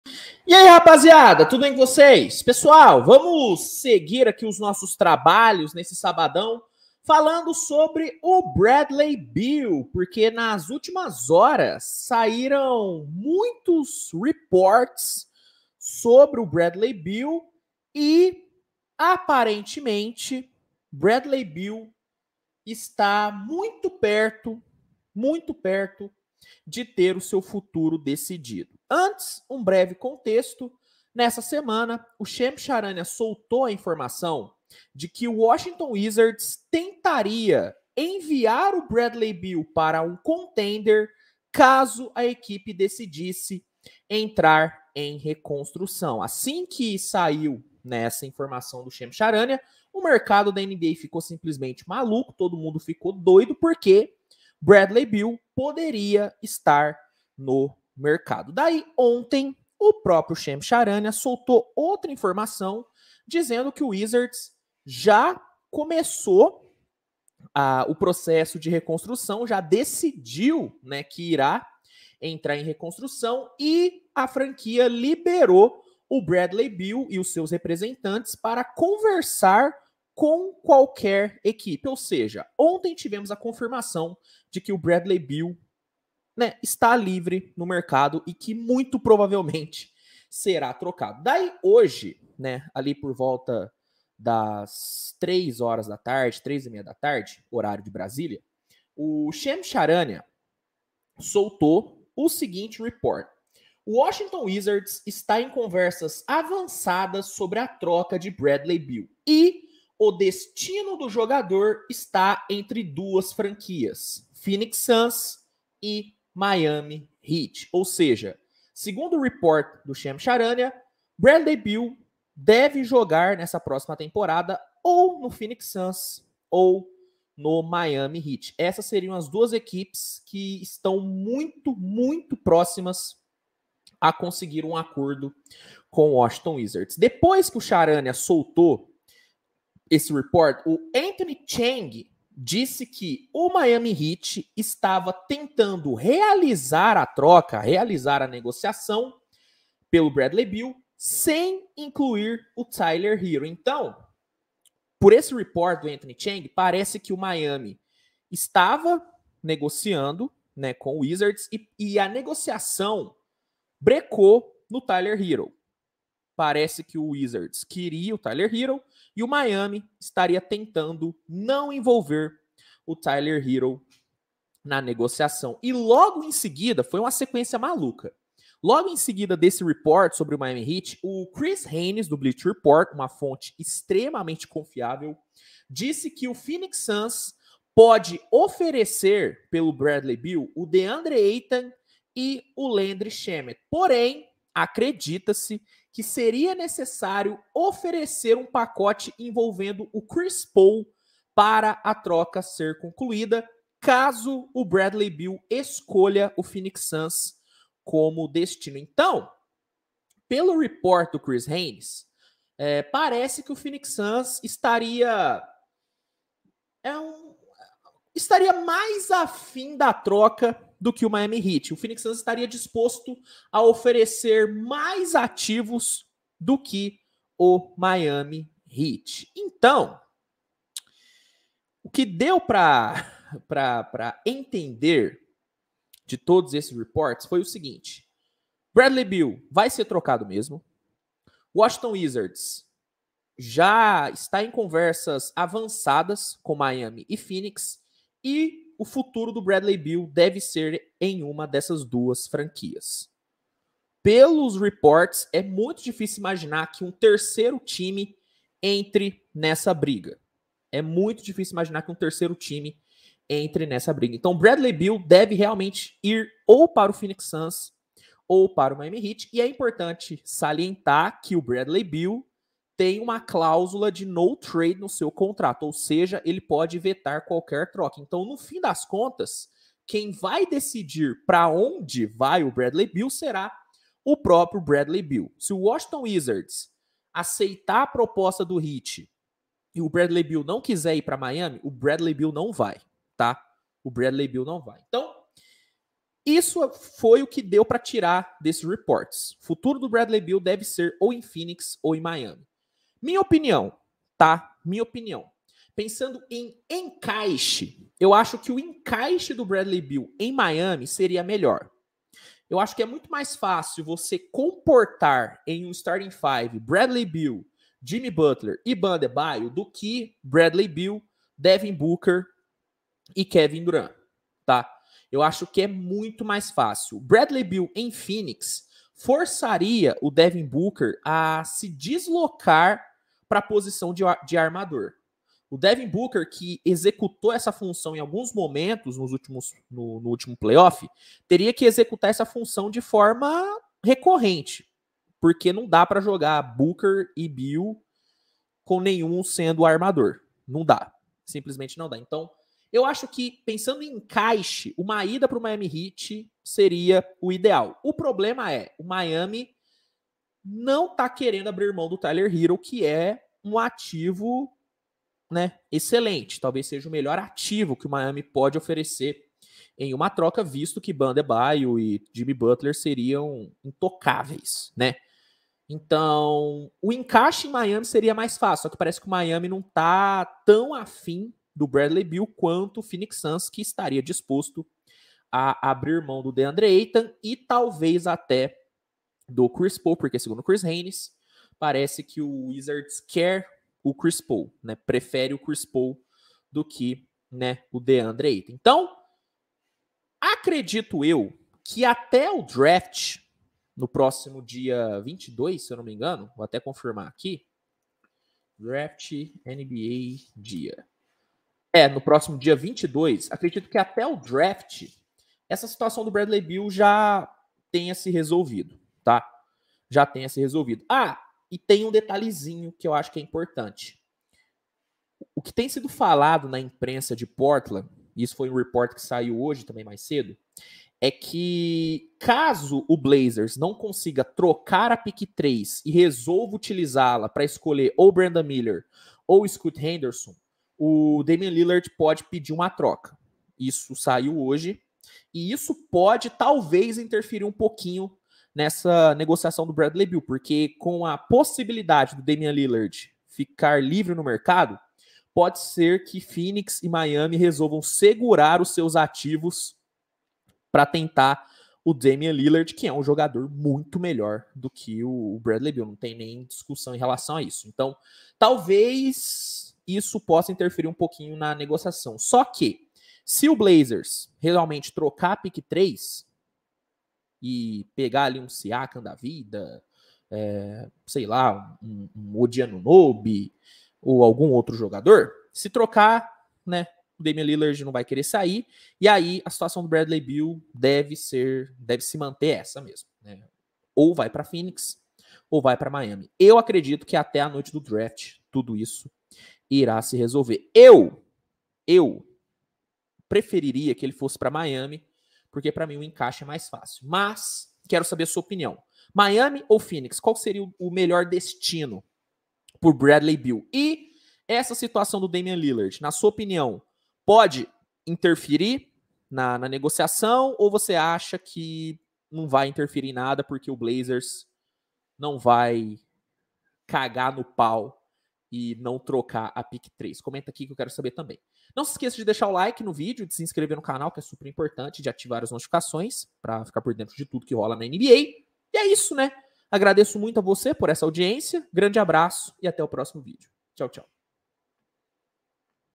Phoenix e Miami são FAVORITOS em TROCA por Bradley Beal - ENTENDA O CASO !
Segundo os últimos reports, o futuro de Bradley Beal deve ser ou o Phoenix Suns ou o Miami Heat !